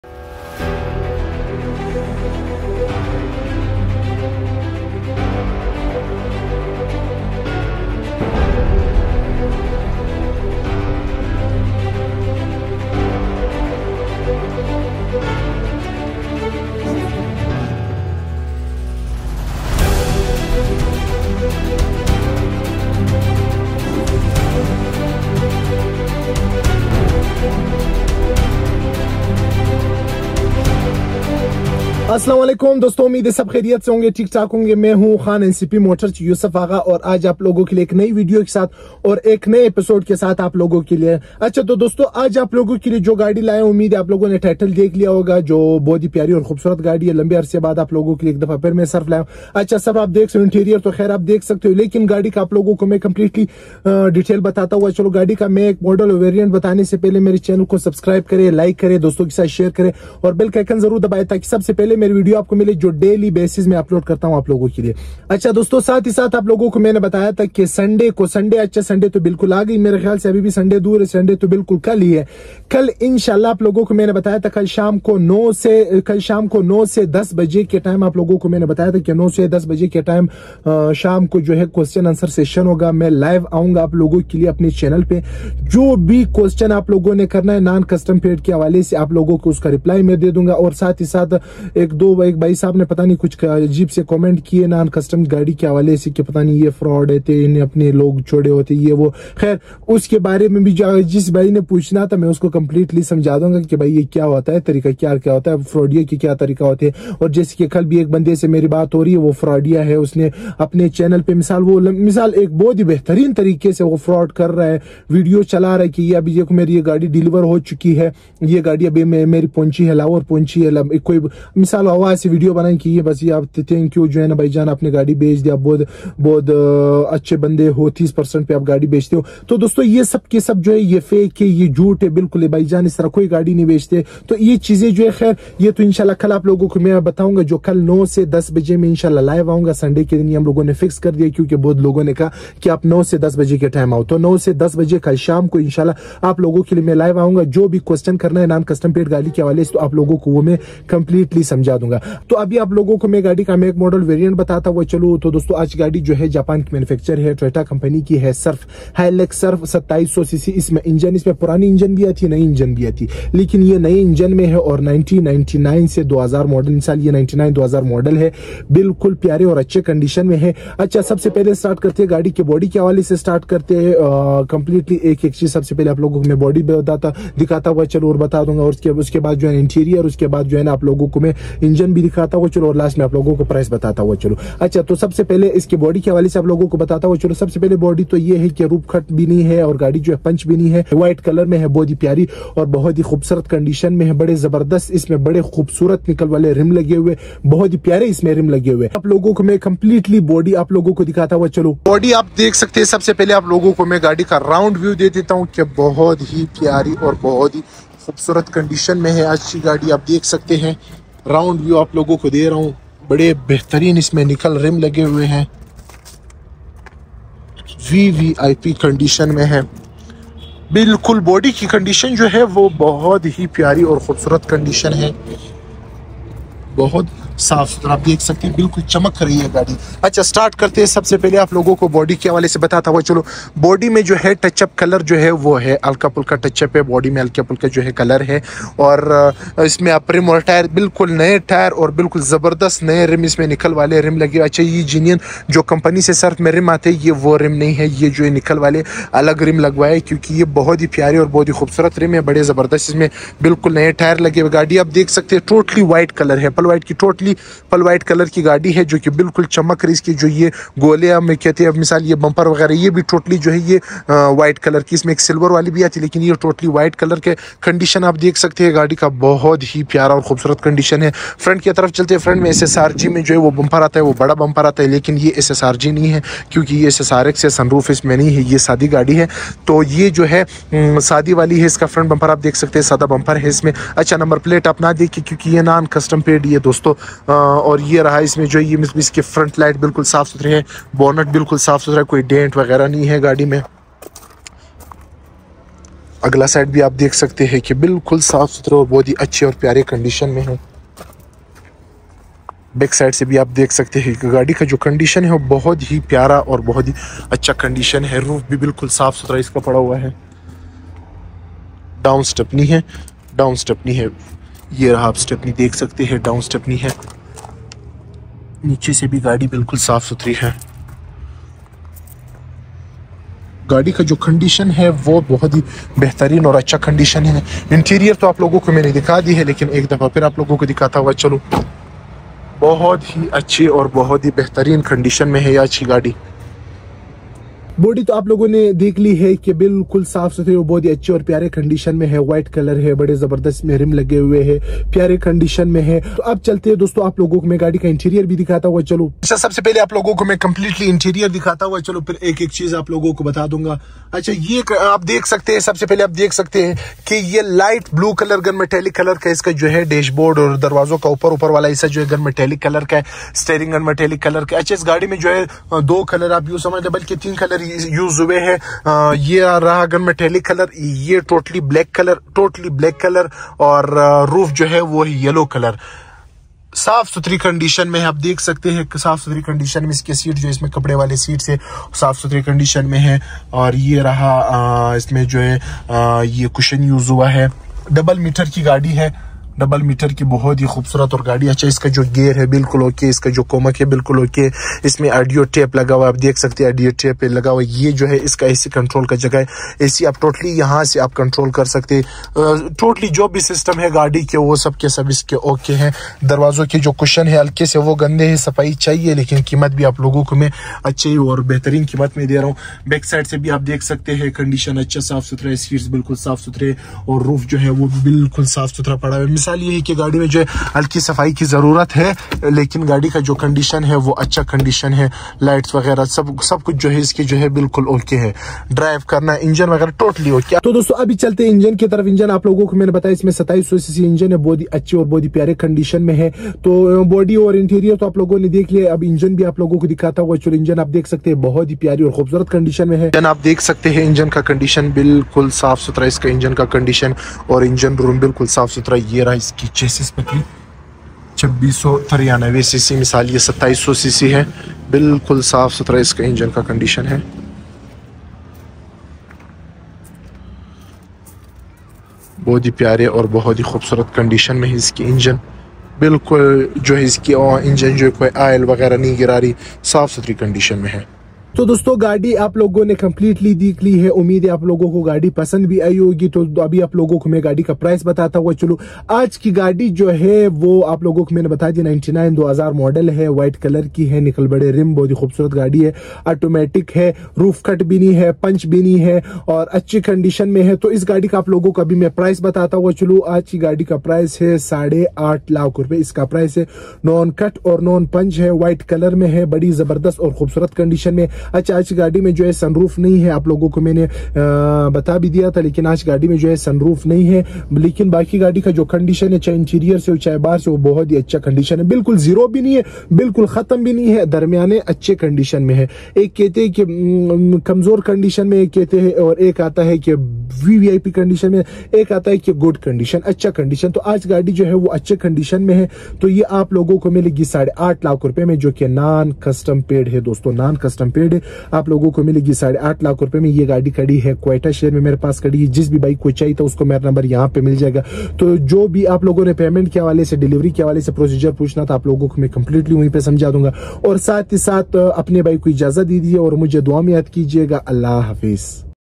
Toyota Hilux surf 2.7 SSR x (2000) NCP | bike k price ma bari gardi | ncp car Quetta | ncp car bazar
Present car is
Toyota
Hilux
Surf
SSR x
Engine 2700
Model 2000
Colour white
Location Quetta
Price 8.5 lac

Every type of non custom paid cars are available

Delivery available to all Pakistan with extra charges